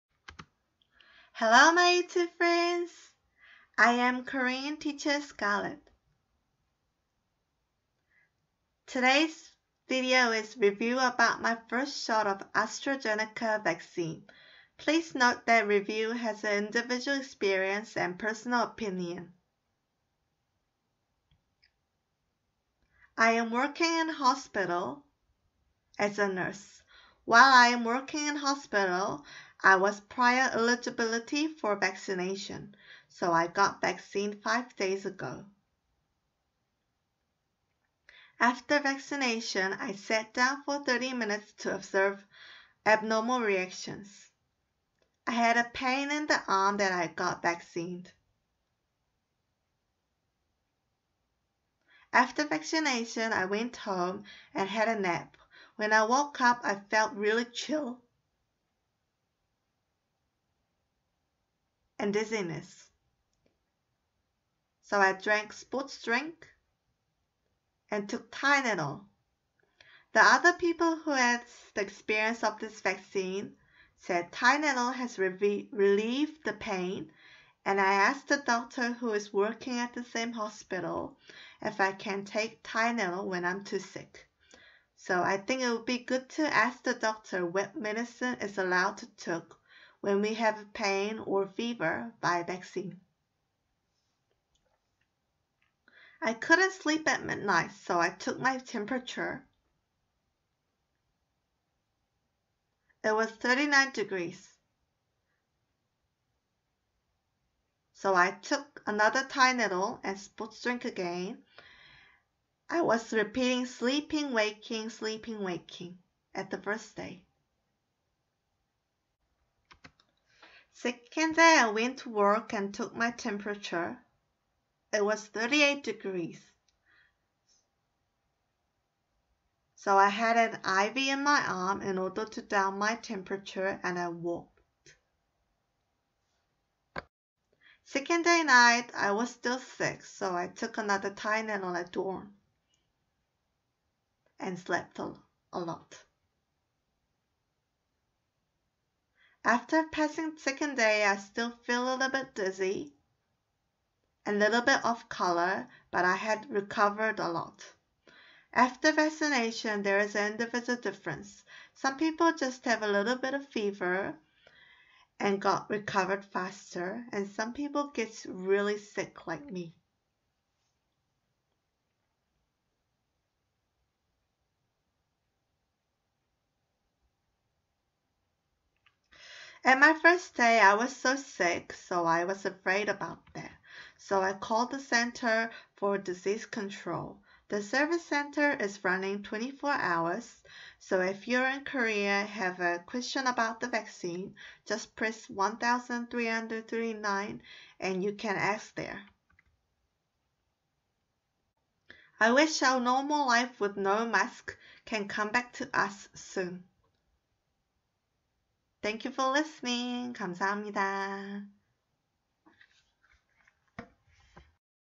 Review about my first shot of AstraZeneca Vaccine-(아스트라제네카 백신 후기 영상입니다 English version)
I am working at hospital as a nurse. I was a prior eligible for vaccination so I took my vaccine 5 days ago.